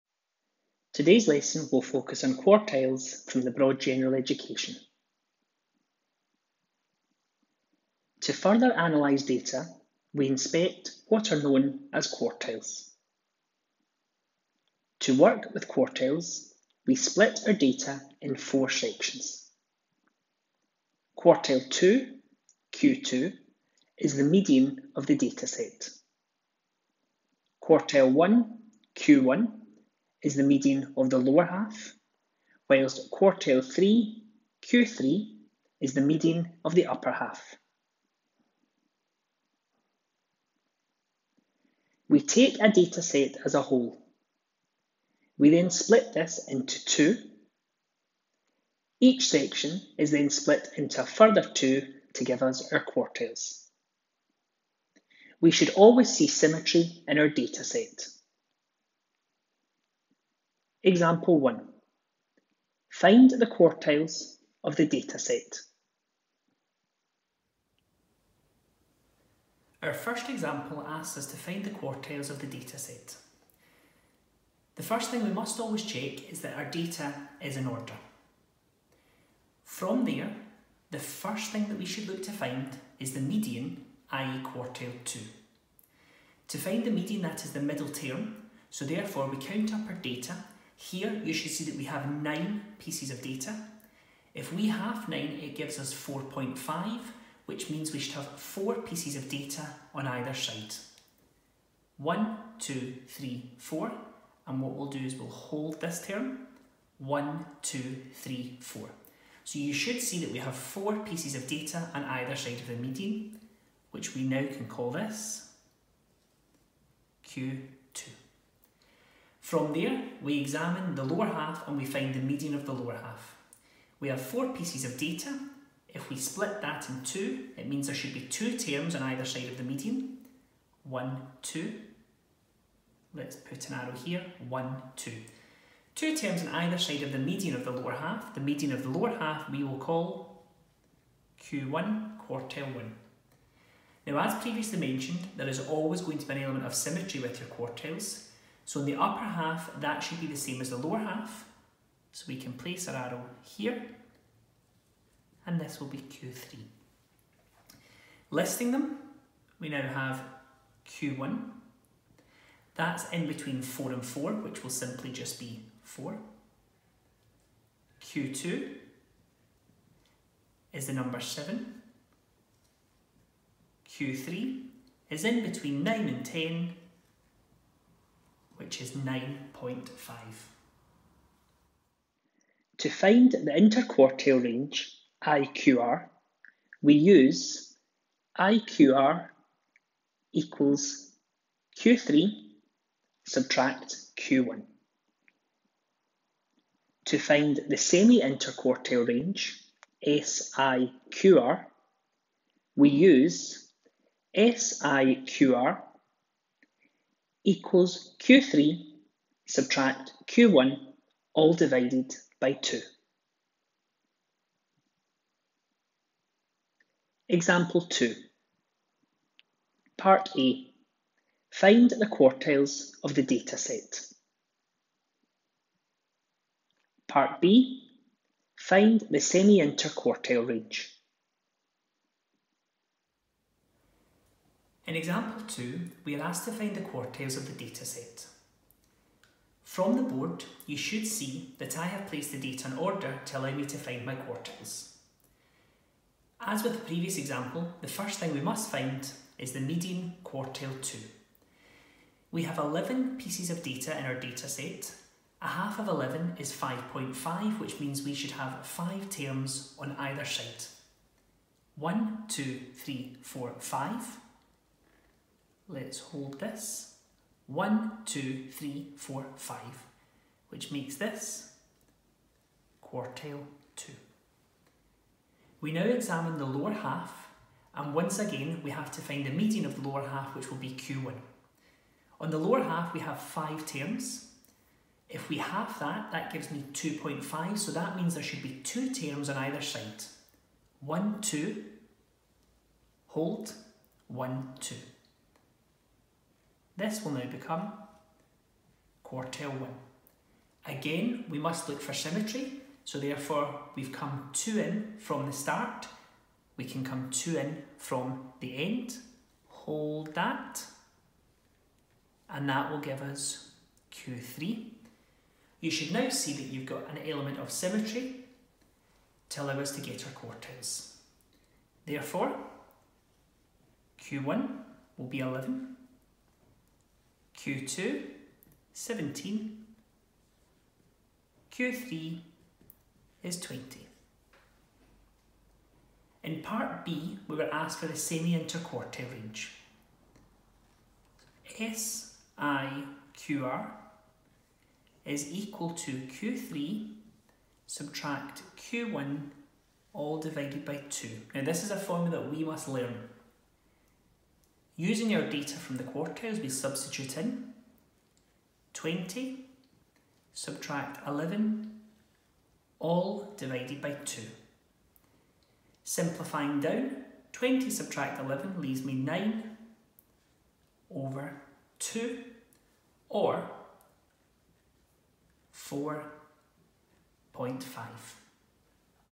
BGE | 2.13. Statistics 2 | 2.13.2 Quartiles
A video explaining the definition of quartiles within a data set and how we can find these.